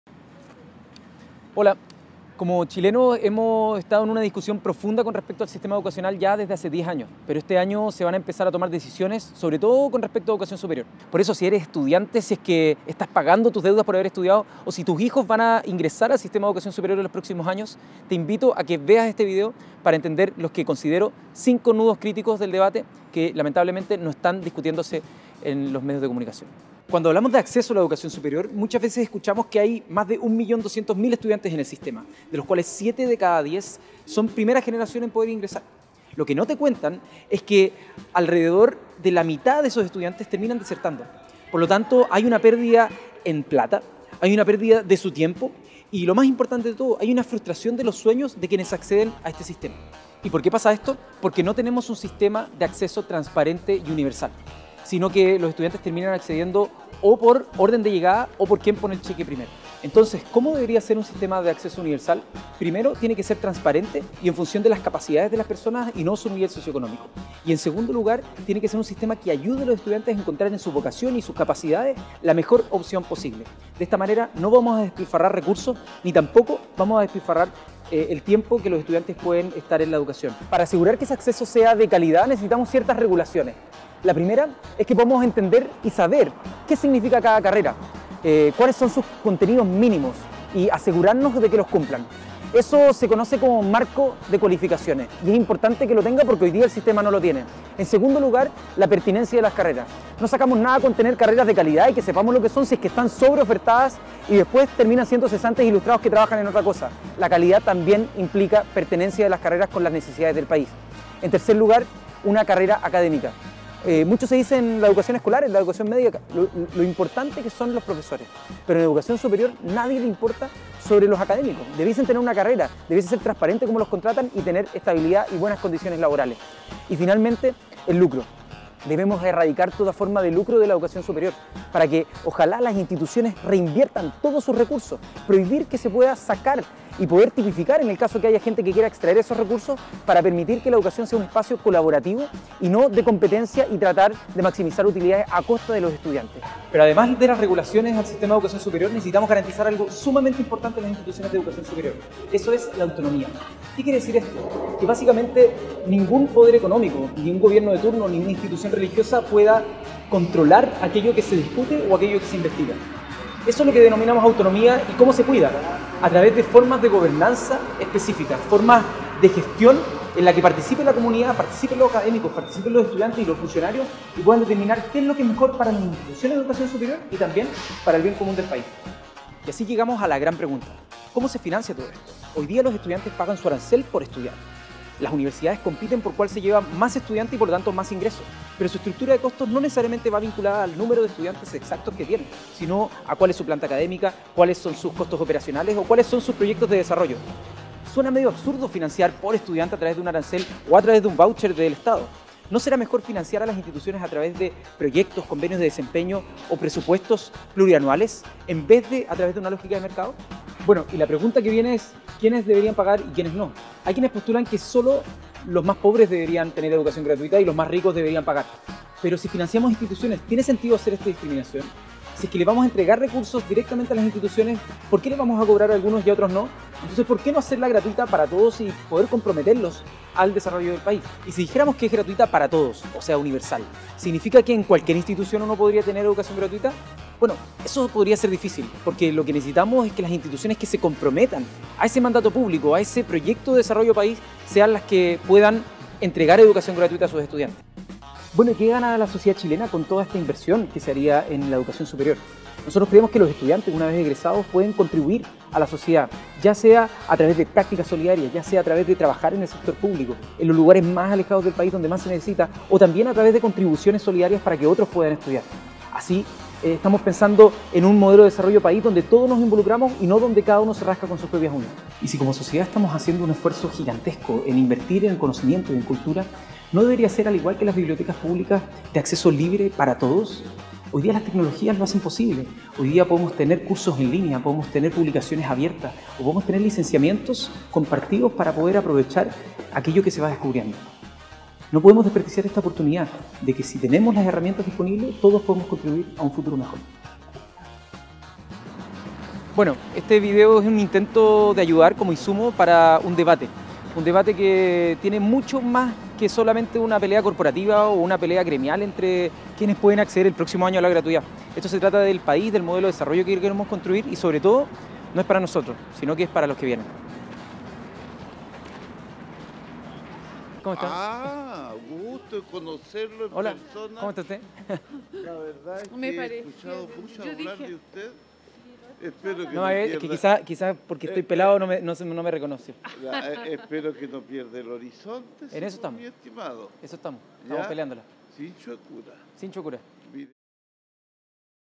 Cinco nudos críticos en el debate de Educación Superior
Con este video intentamos entregar un insumo para el debate en educación superior. Compártelo y comenta acá abajo los 5 nudos críticos o alguna inquietud adicional sobre el debate.